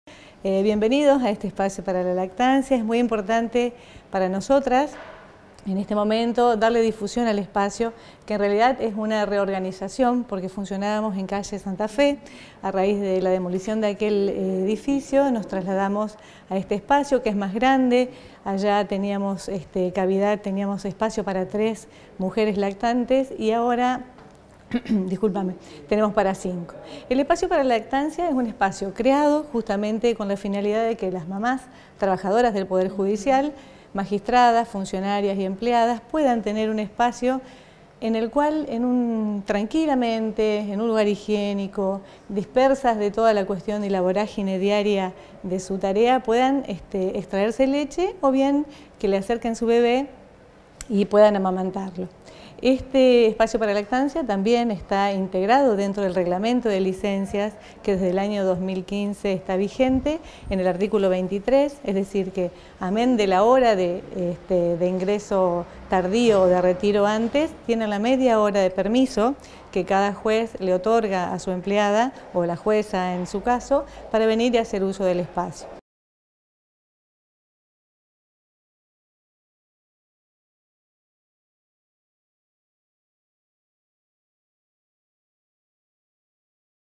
Así es el "Espacio para la Lactancia" que funciona en Tribunales de Paraná
El nuevo espacio para la Lactancia funcionará ahora en el edificio de Tribunales, a cargo de la Oficina de la Mujer del STJ. Está pensado para todas las trabajadoras del Poder Judicial que se encuentran en periodo de lactancia.